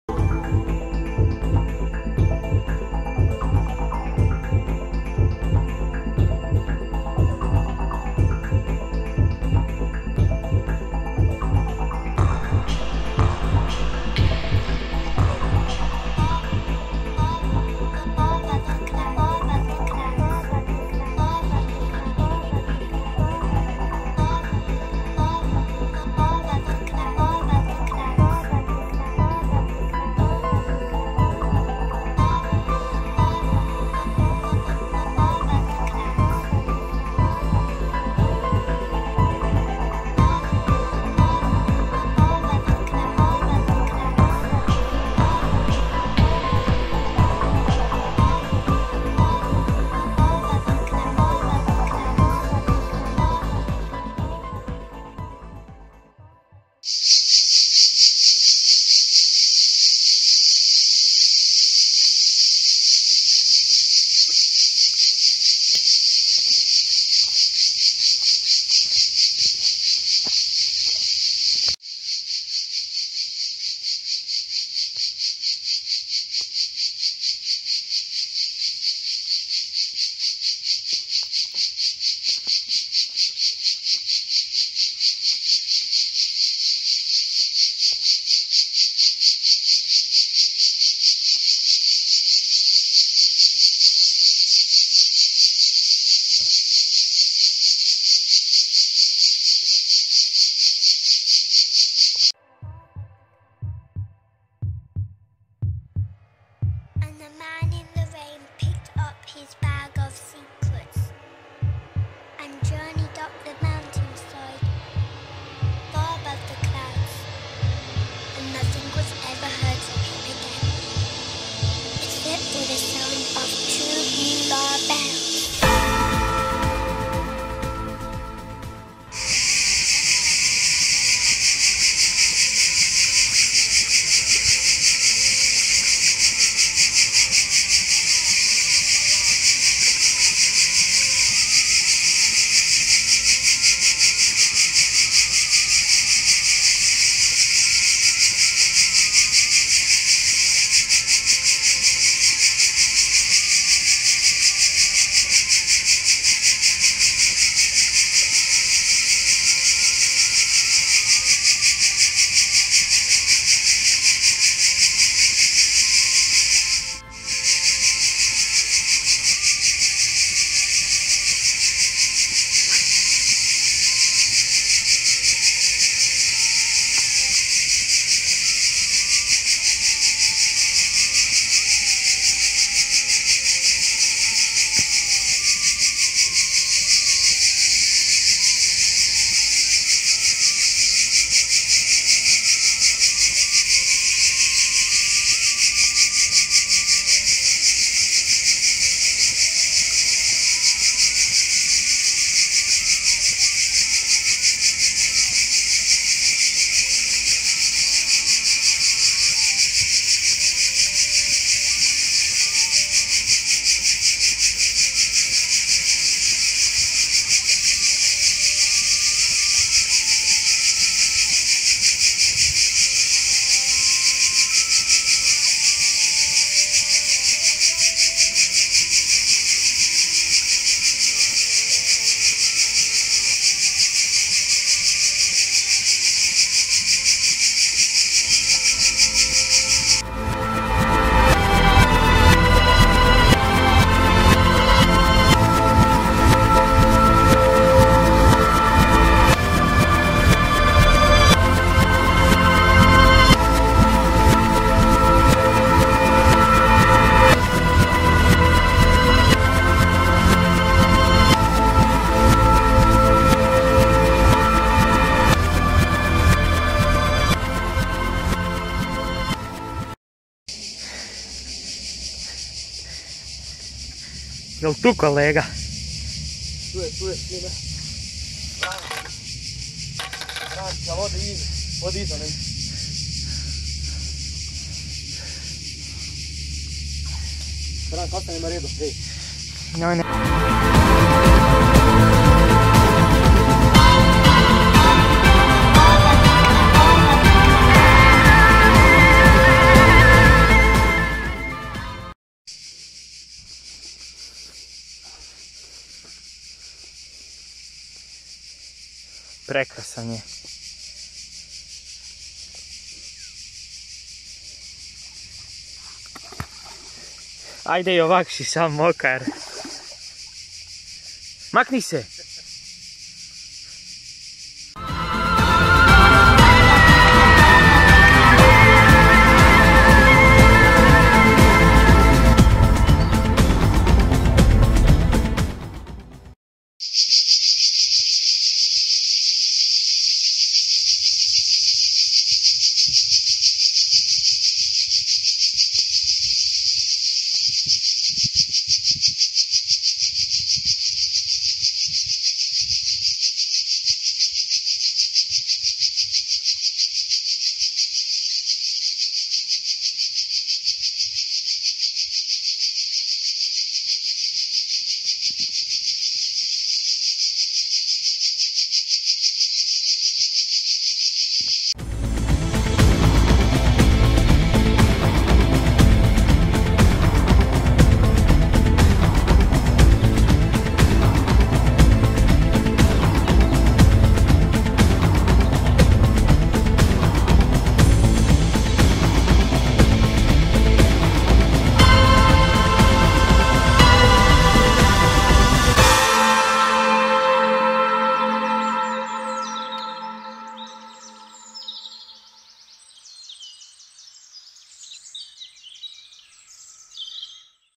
PECANJE ŠARANA NA VRANSKOM JEZERU 2022
Carp team susjedi (tata i sin) fali susjed, prvi put na vranskom jezeru i šaran od 5.5 kg yessssssssssss